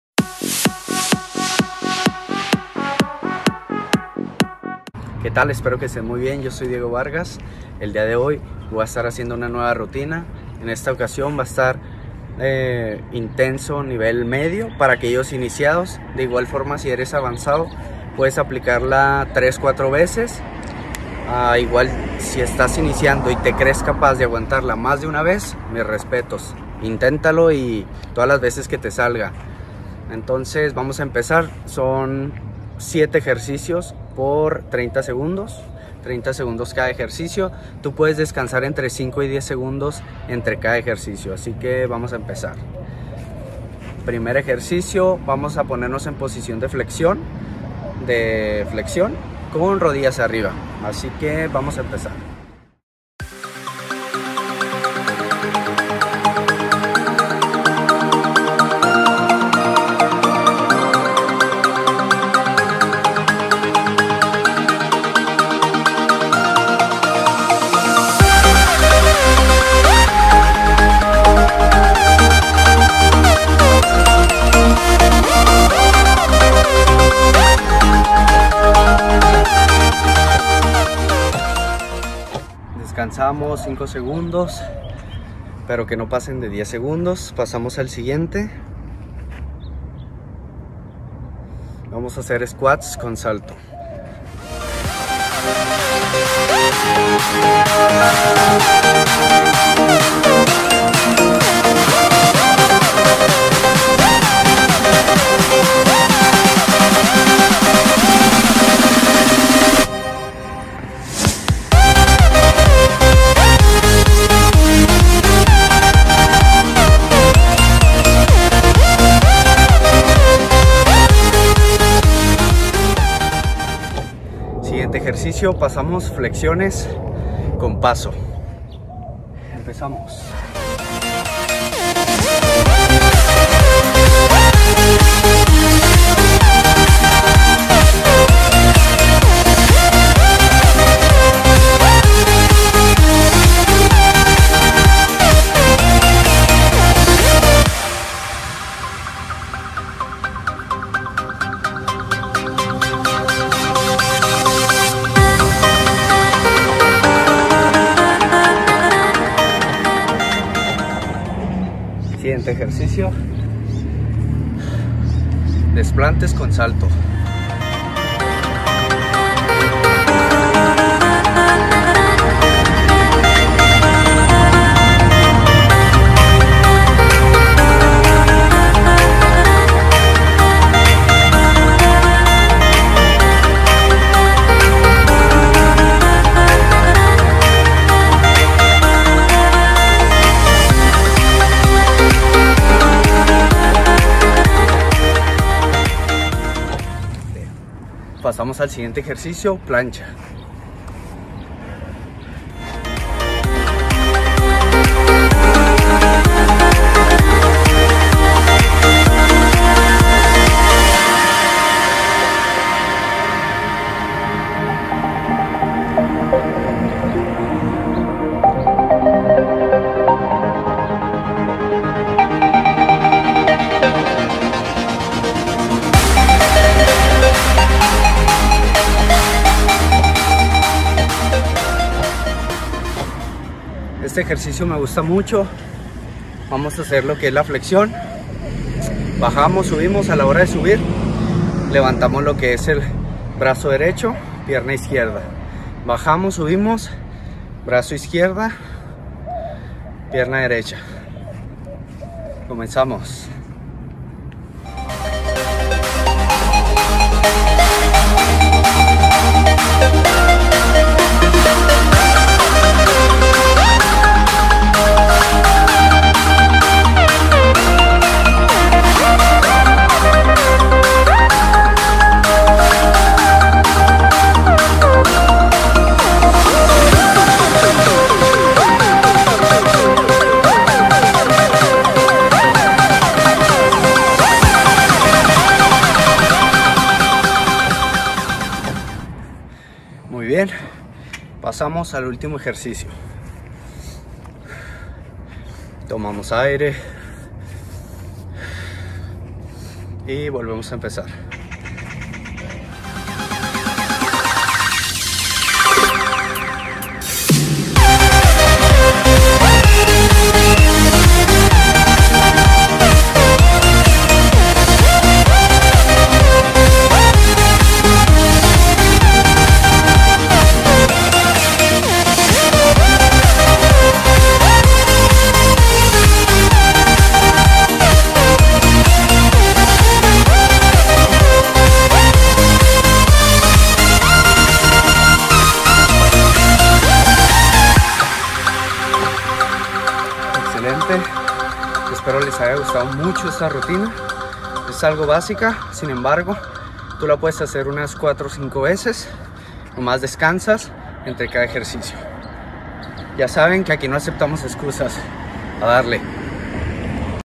Rutina para perder grasa abdominal | Diego V
En este video realizo una rutina para lograr marcar esos abdominales y perder grasa.
Si quieres darle un cambio a tu fisico te invito a que le des like y te suscribas para que no te pierdas ninguna rutina, que por cierto subo dos videos por semana.
Ya sabes que aqui no aceptamos excusas!
A darle!